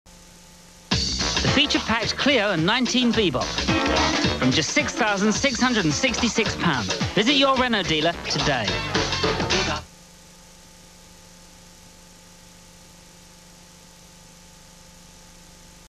Renault Clio & 19 'Be-Bop' car advert Pt.2 of 2 - Broadcast 10th August 1994 (UK)
with voice over by Jools Holland